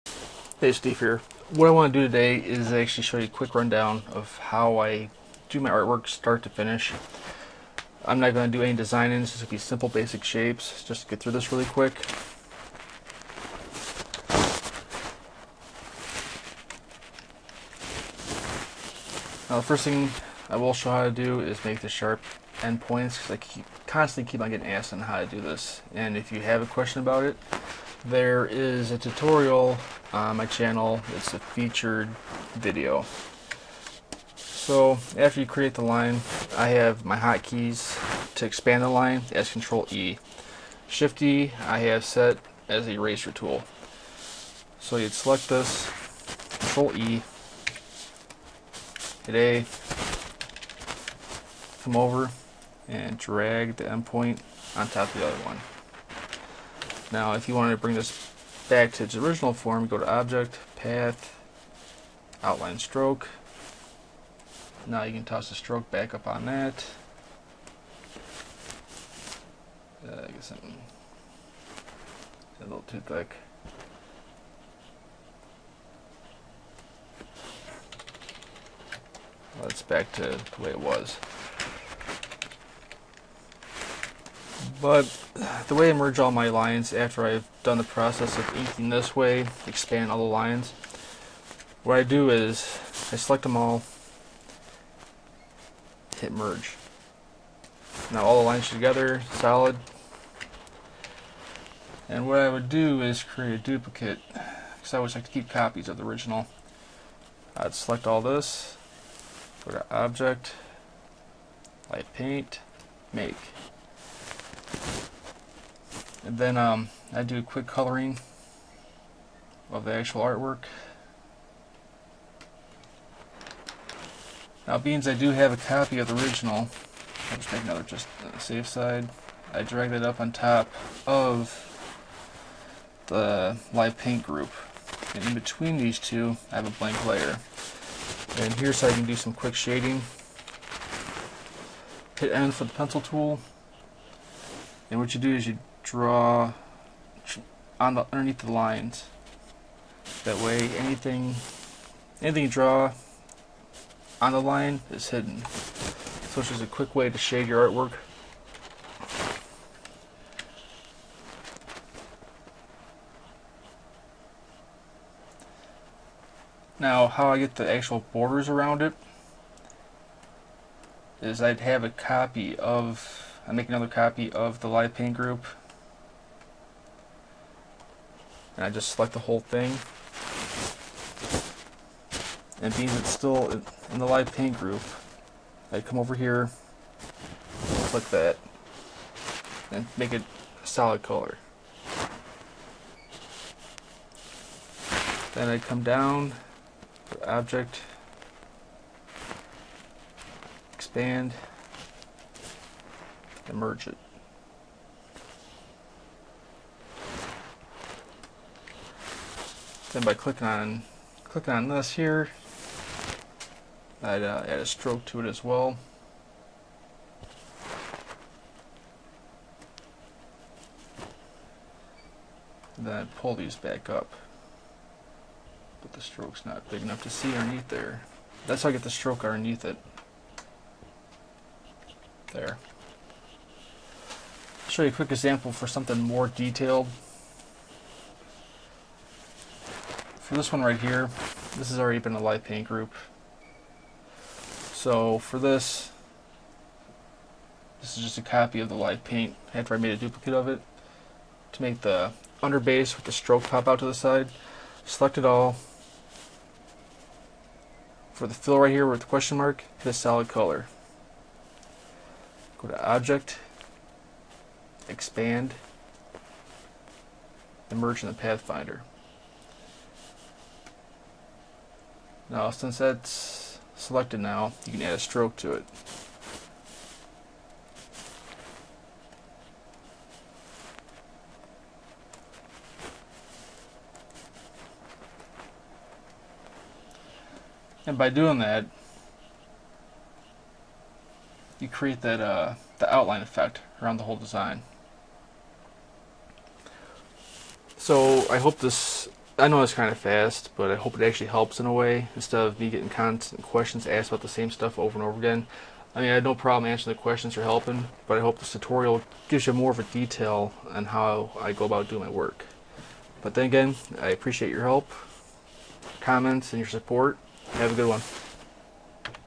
Ultimate inking,coloring, shading, and cell shading tutorial in Adobe Illustrator- Swiftyspade
In this video, I show you how to ink, color, make sharp endpoints, and create an outline stroke around your finished artwork. This method is used by using livepaint, merge option, expand option, and pencil tool for the shading of your work. You also learn a quick way of shading your work that can also work for cell shading in Illustrator. Sorry for the static in the audio, the mic is sensitive.